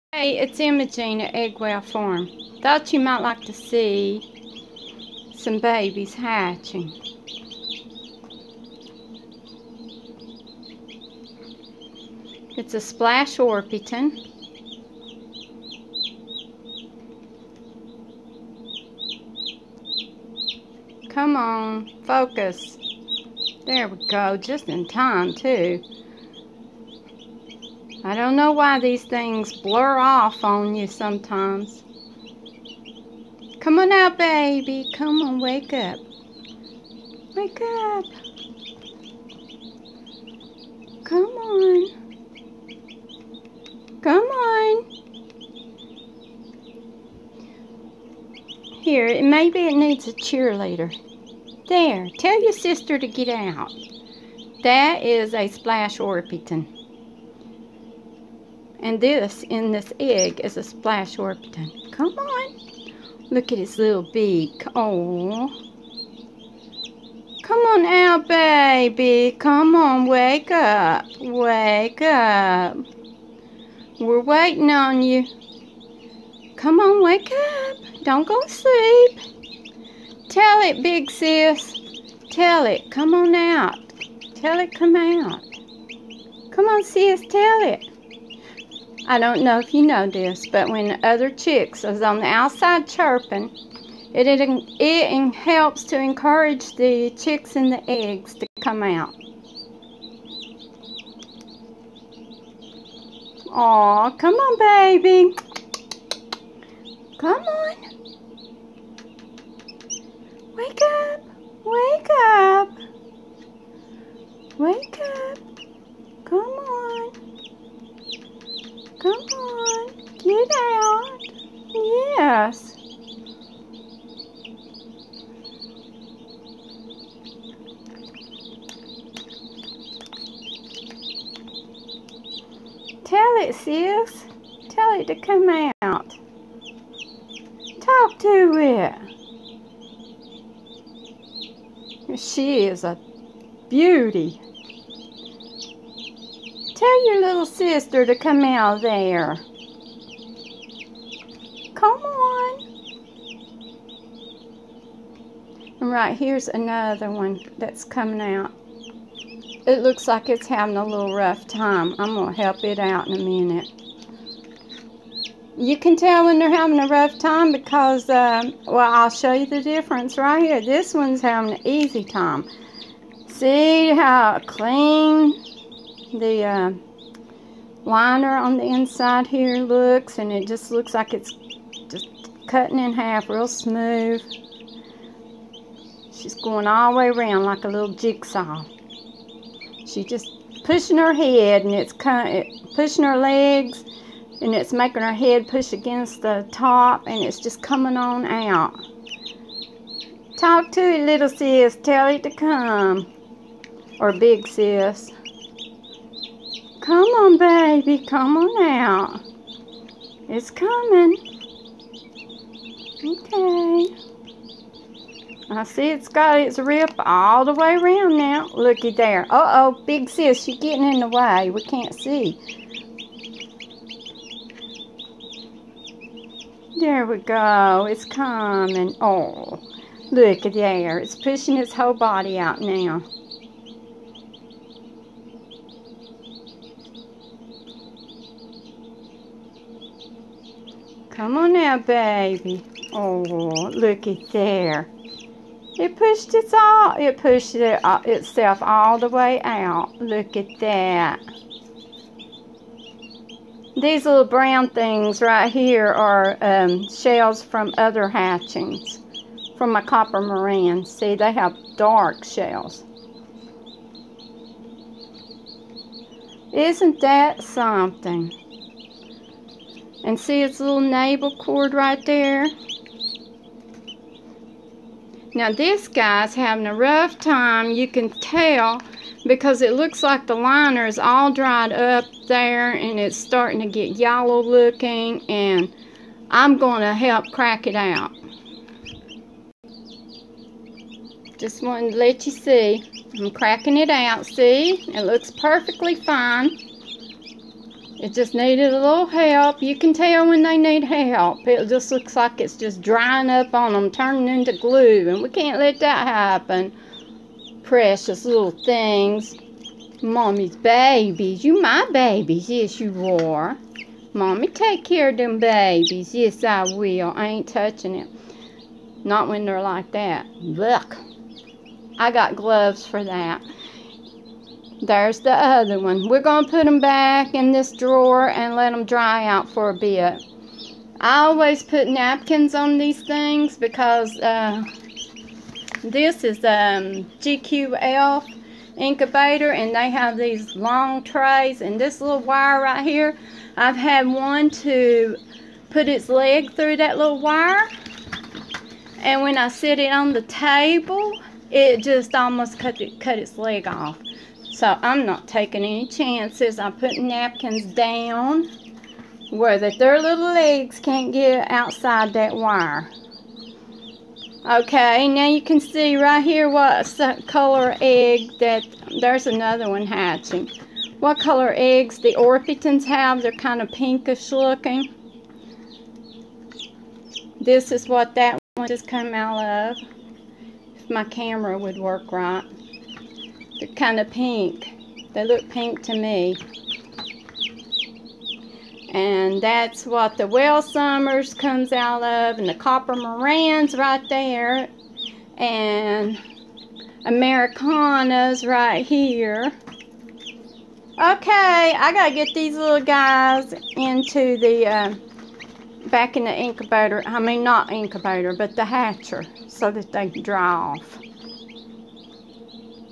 Chick Hatches
This is a video that shows a chick hatching out of its shell. It is a Splash Orpington